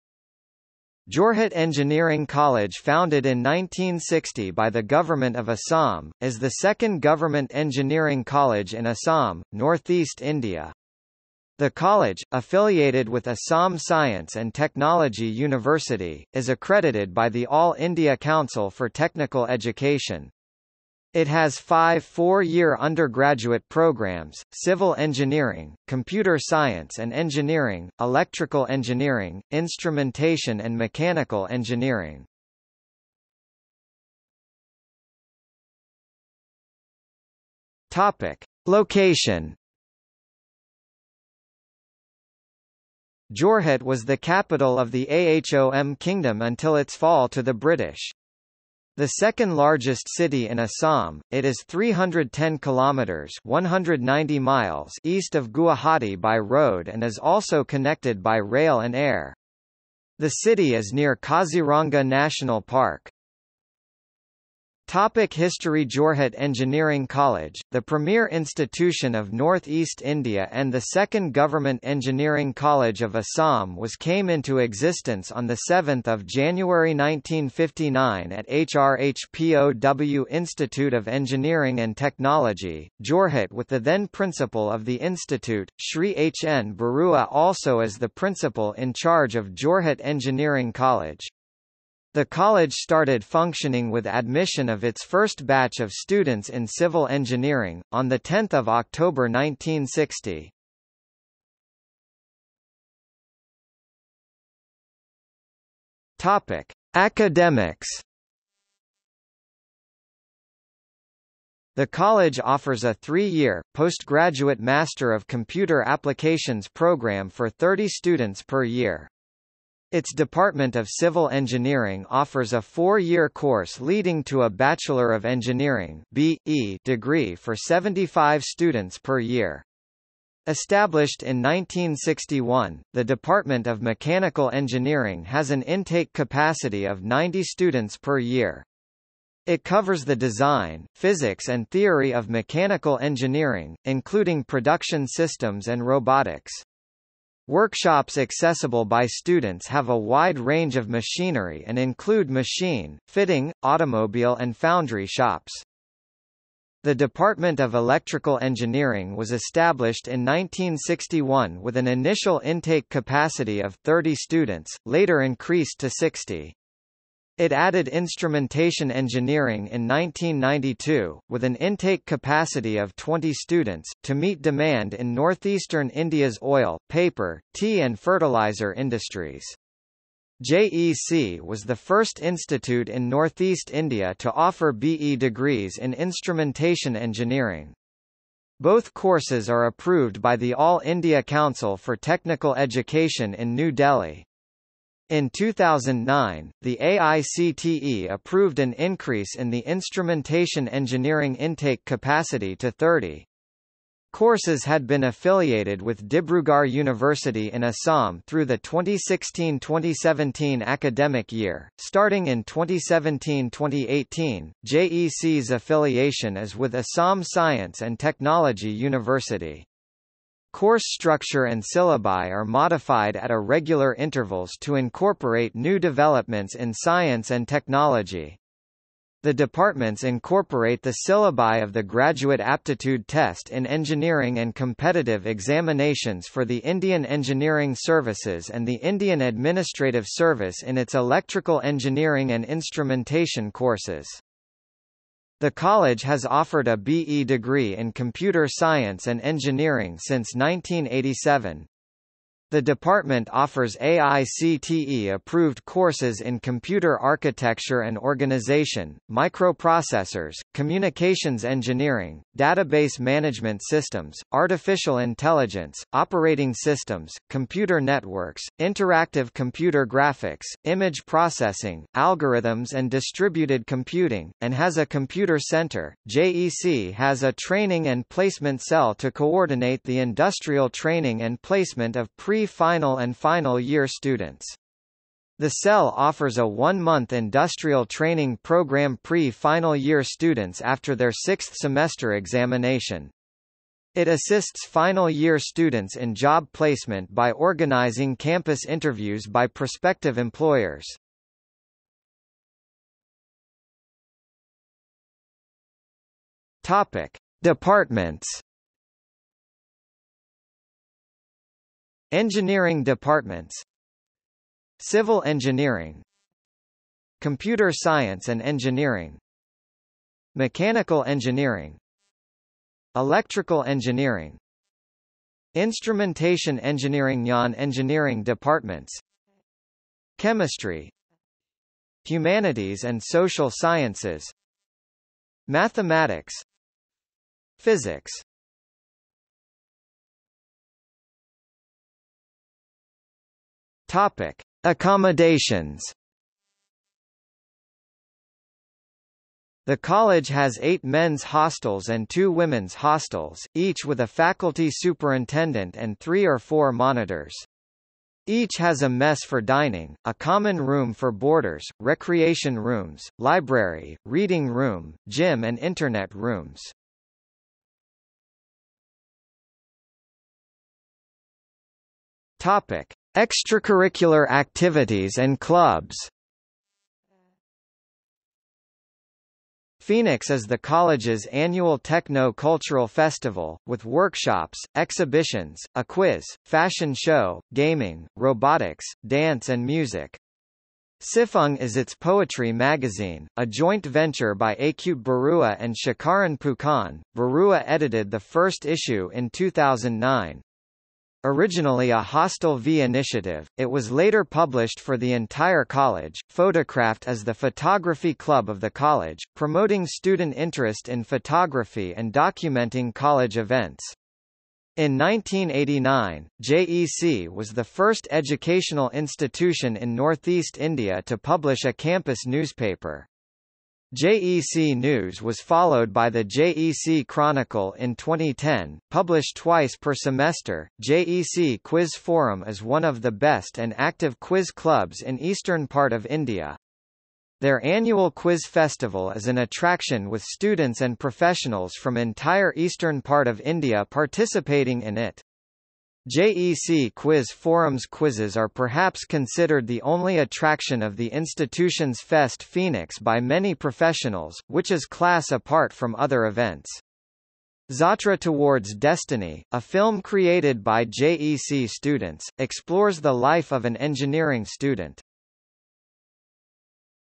Jorhat Engineering College | Wikipedia audio article
00:00:34 1 Location
00:01:04 2 History
00:01:42 3 Academics
00:05:03 4 Departments
00:05:40 5 Accommodations
00:06:08 6 Extracurricular activities and clubs

- Socrates

SUMMARY
Jorhat Engineering College founded in 1960 by the government of Assam, is the second government engineering college in Assam, northeast India. It has five four-year undergraduate programs: Civil Engineering, Computer Science and Engineering, Electrical Engineering, Instrumentation and Mechanical Engineering.